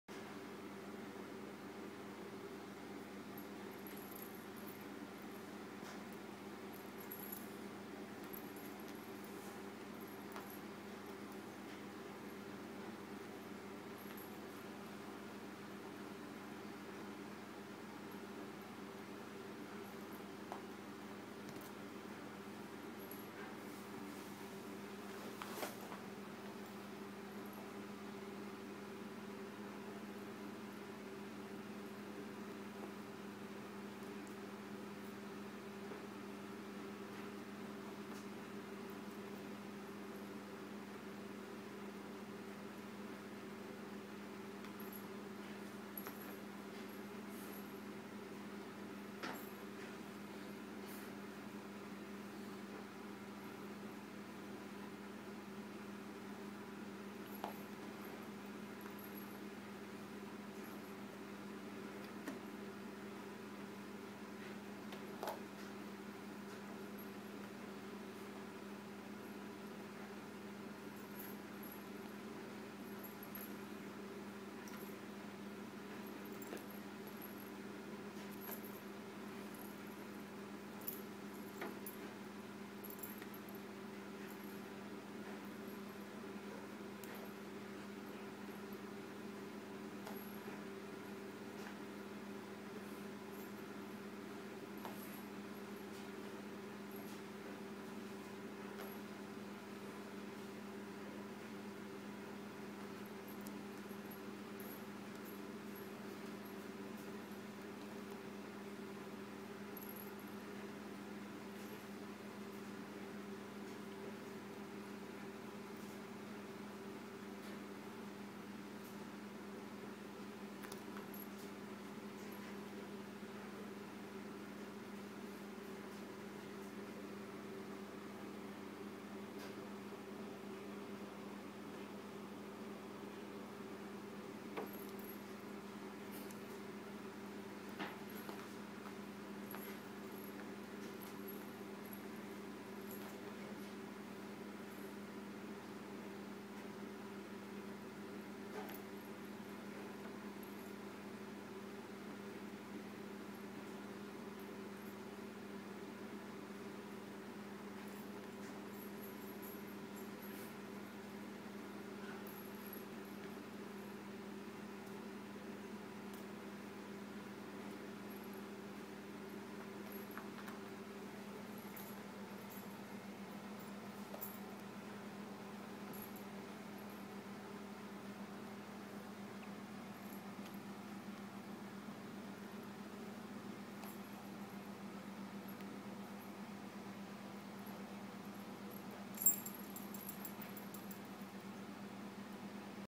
Lazy Cat Hunts The Noodle On His Back
Malcolm is a very lazy cat when it comes to hunting.